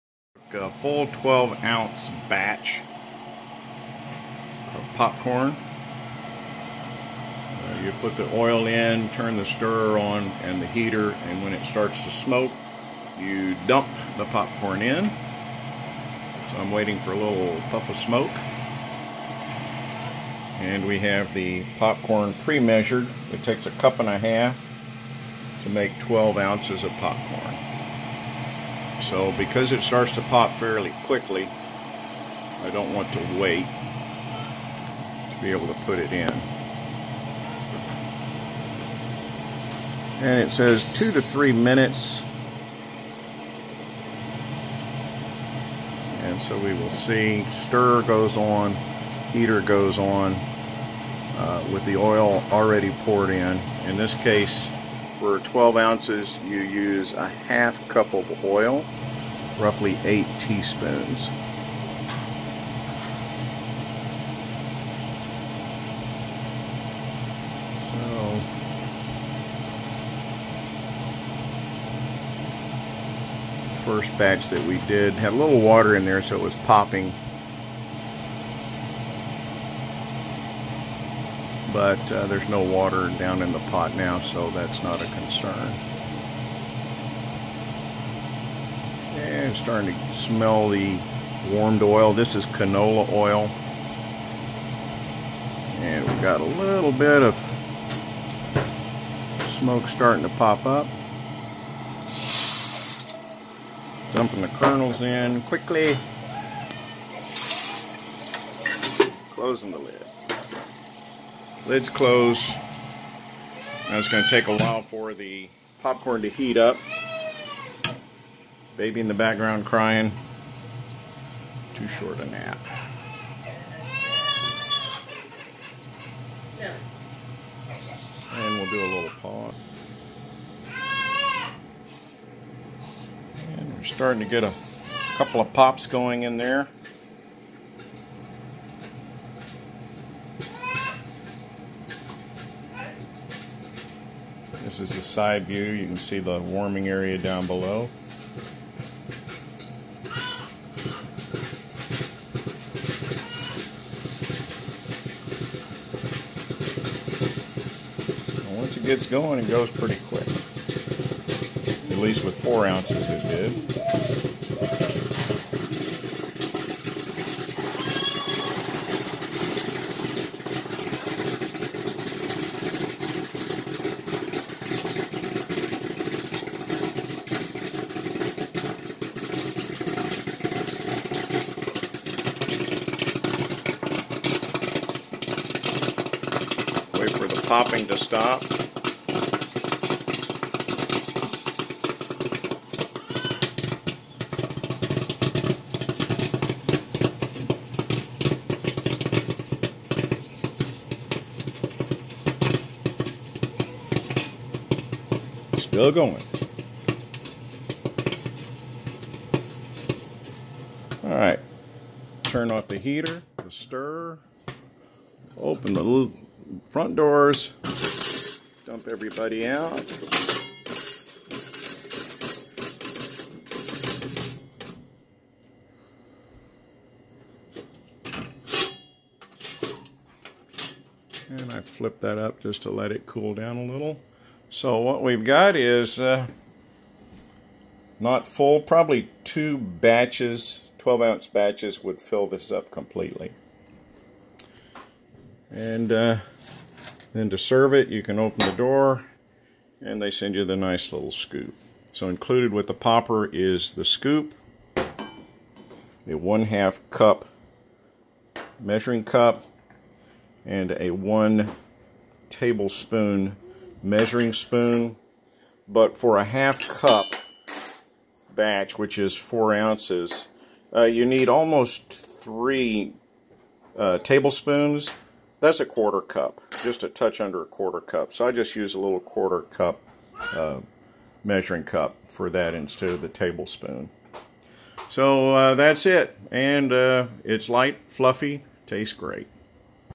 Popping 12 oz batch
Our 2nd batch of popcorn was 12 oz and popped very quickly and tasted good.